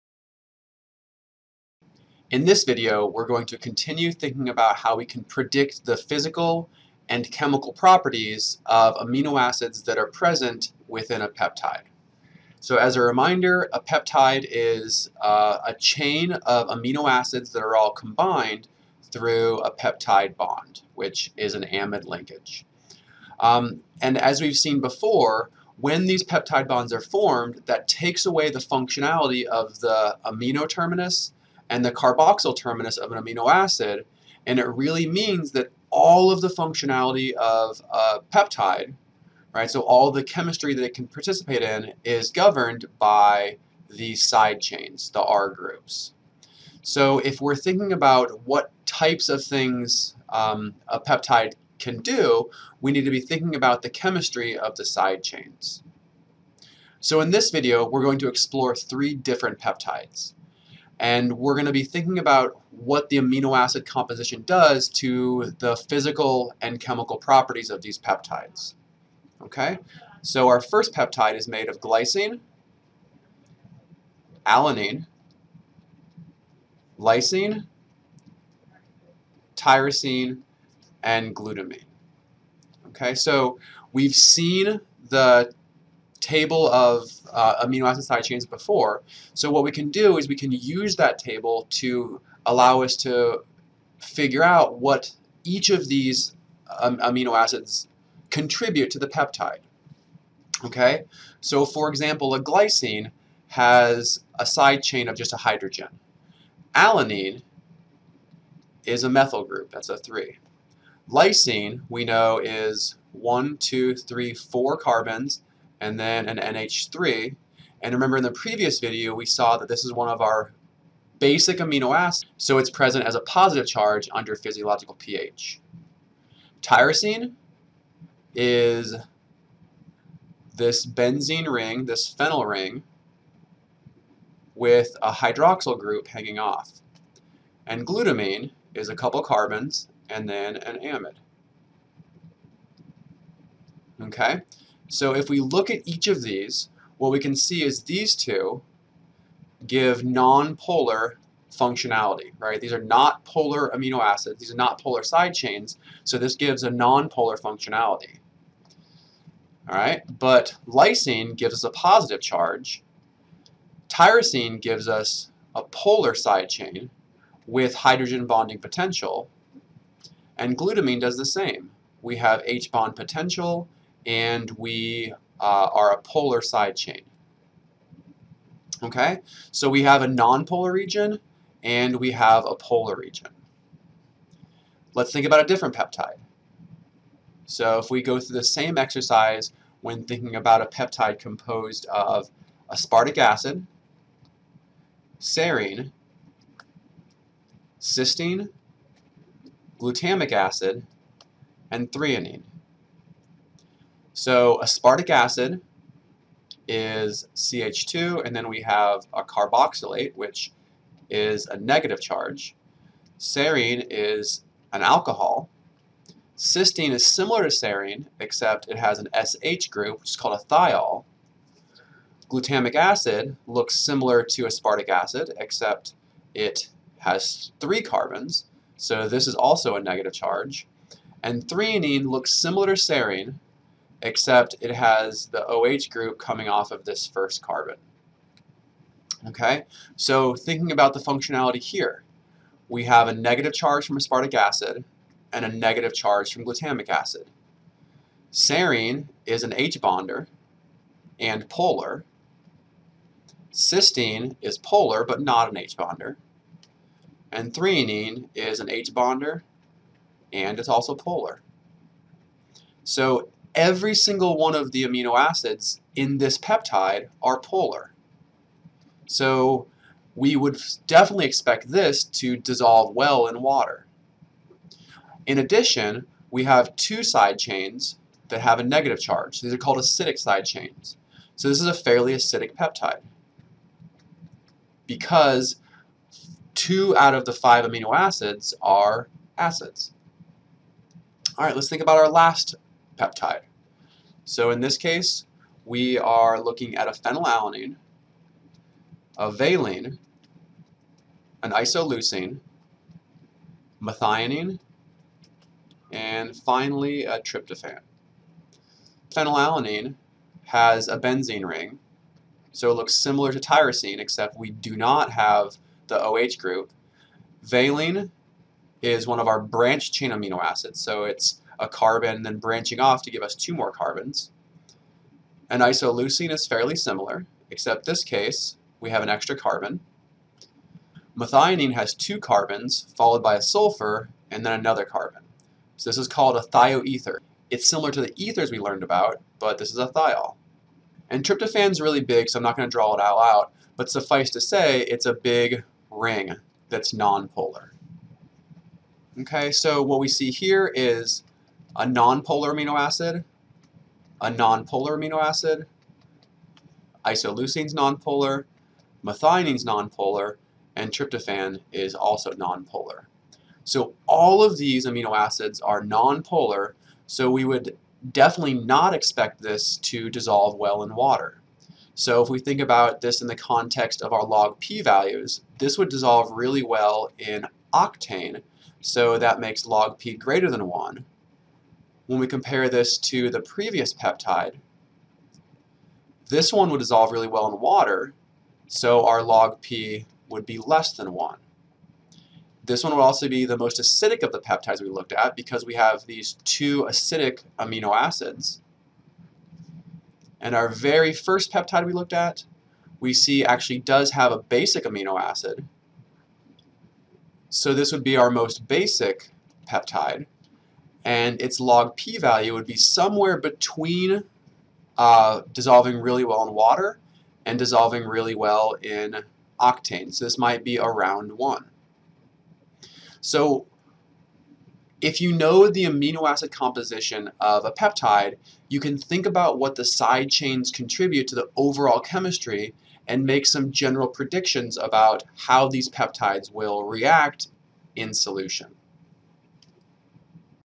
Peptide Composition
This video explores how the order of amino acids can influence the physical and chemical properties of a peptide.